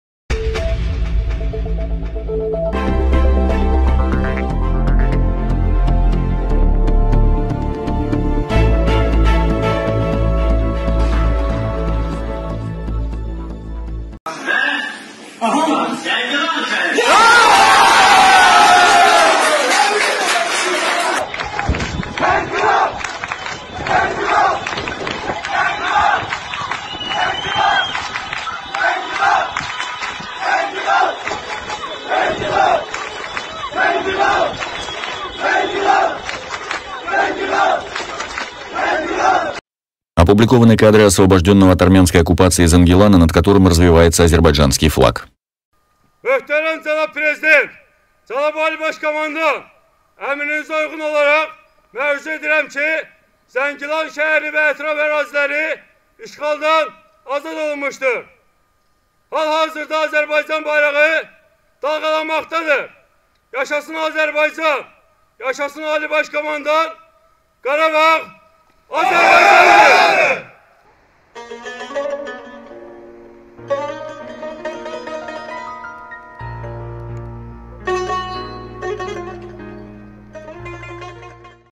Над освобождённым от оккупации Зангиланом развевается Азербайджанский флаг
Опубликованы кадры освобождённого от армянской оккупации Зангилана, над которым развевается Азербайджанский флаг.

Mus: Ana